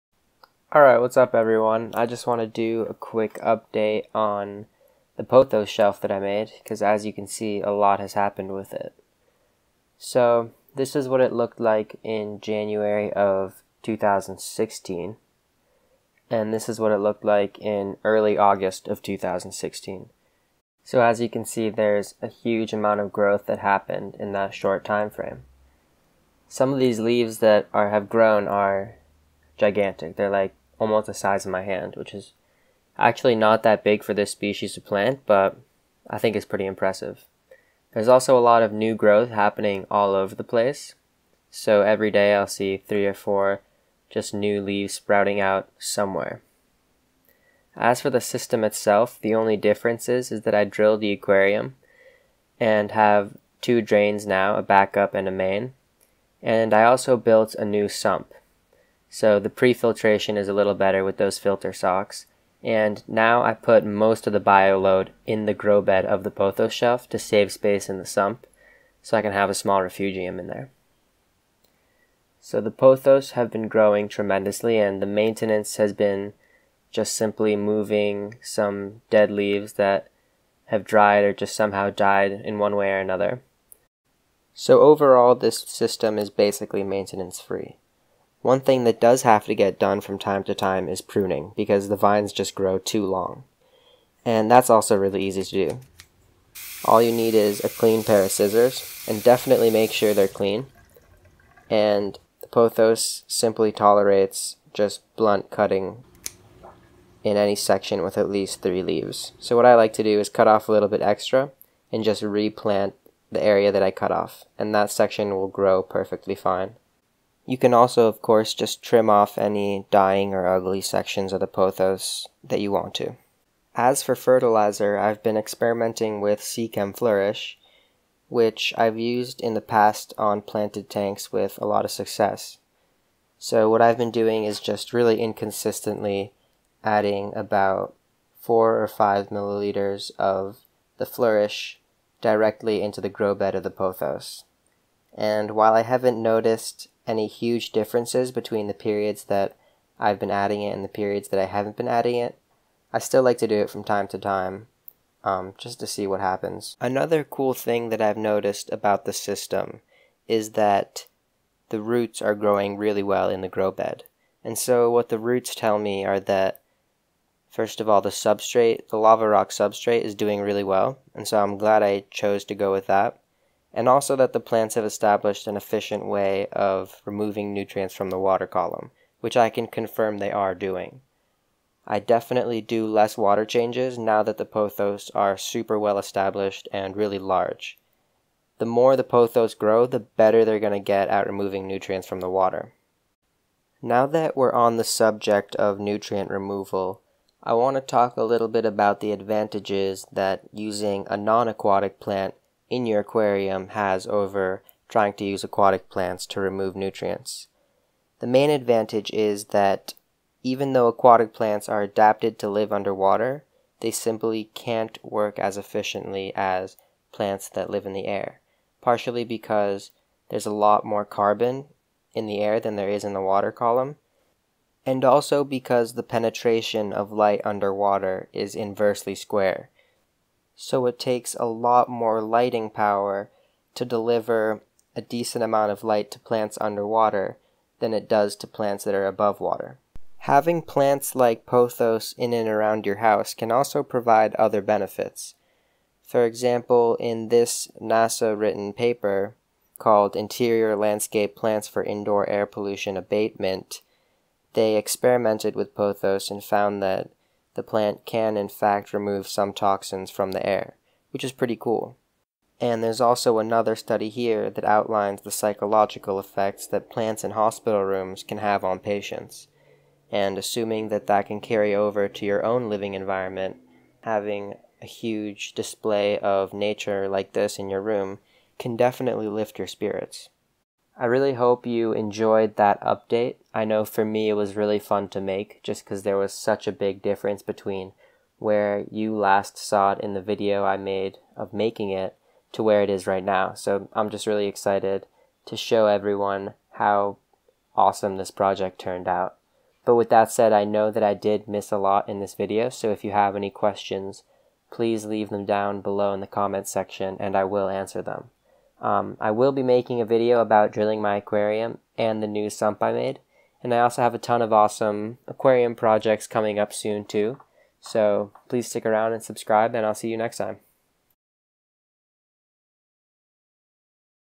Pothos Update
I know I forgot to mention a lot, I have so much to say about this project but I didn't have much time to throw this video together and I wanted to get it done quickly.

I typically light for 10-14 hours a day. It works well.